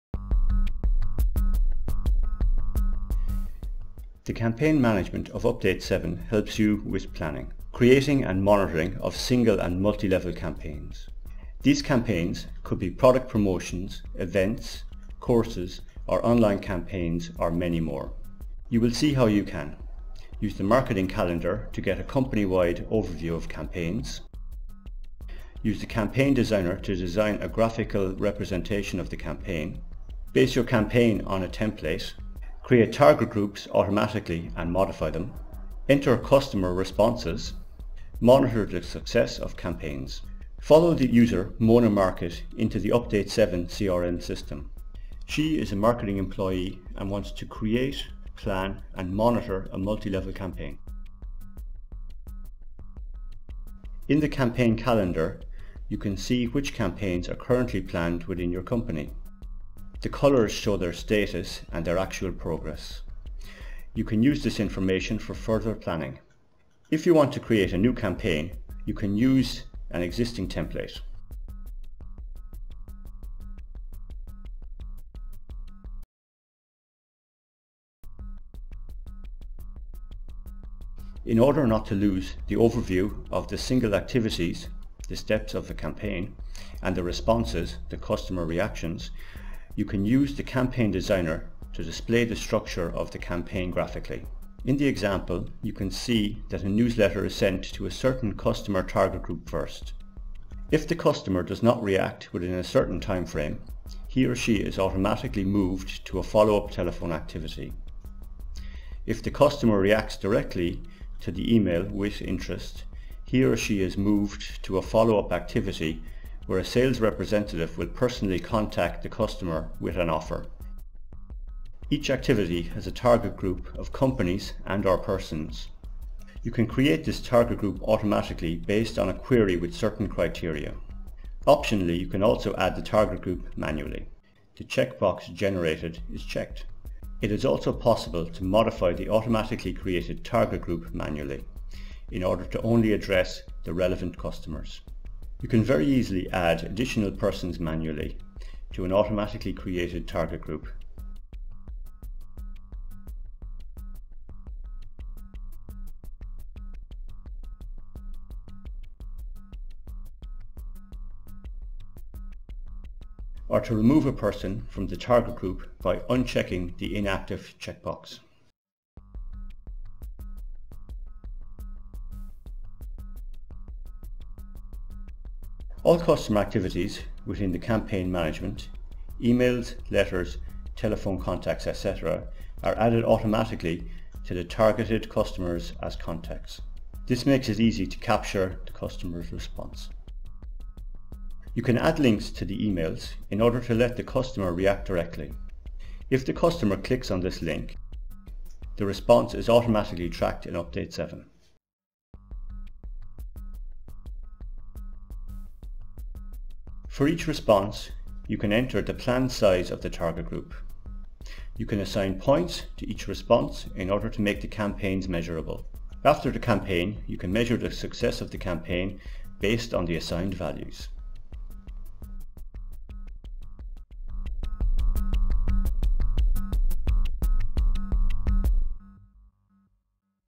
CRM Process: Campaign Management in update CRM (English)
Campaign Management assists you with the planning, execution, and monitoring of single and multi-stage campaigns. A campaign consists of several activities that follow one another. This way, you can map an entire event, from the invitation to the event itself, as well as follow-up activities after the event.

- Easy handling of campaigns via planning, definition of target groups, execution, on through to monitoring
- Transparency with respect to planned, executed, and ongoing campaigns
- Automatic response recording